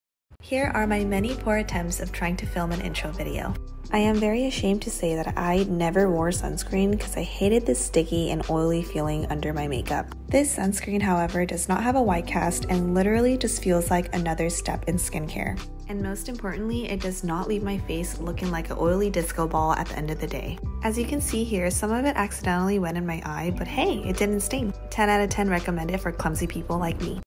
Round Lab Birch Tree Line Sunscreen Review
ABSOLUTELY the best sunscreen ever!
Find out why this Sunscreen is the THE best selling sunscreen in Korea.

A moisturizing sunscreen protects your skin from harmful UV and replenishes and hydrates your skin. Protects skin from harmful UV rays with SPF50+, PA